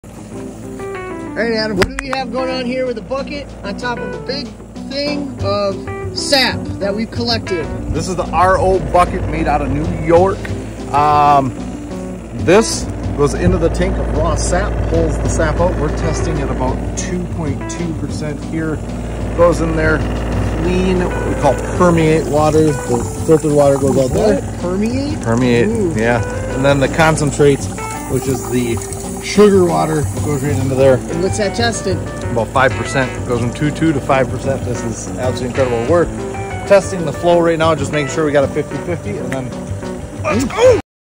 Unlocking the Magic of Maple Syrup: Reverse Osmosis Bucket Filter System Revealed
This little reverse osmosis in a bucket is sure to speed things up when cooling sap into maple syrup.